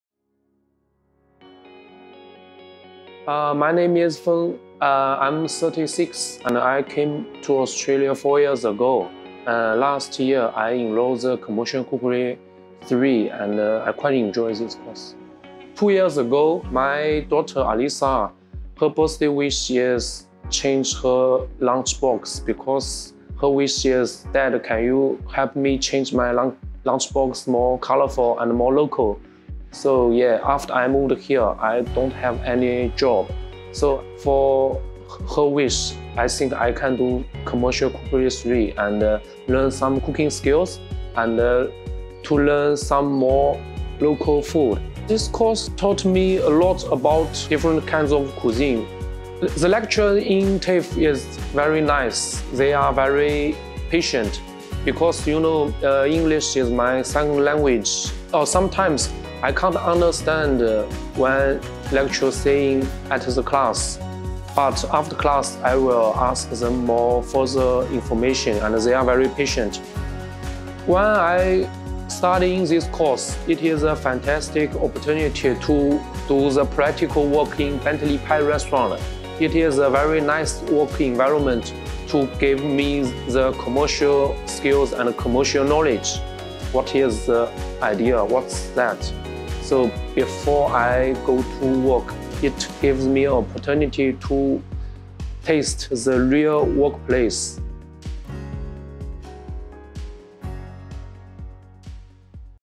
Feng - WA Cultural Diversity Training Award Winner 2022
Hear from Feng, WA's Cultural Diversity Training Award winner and SM TAFE student.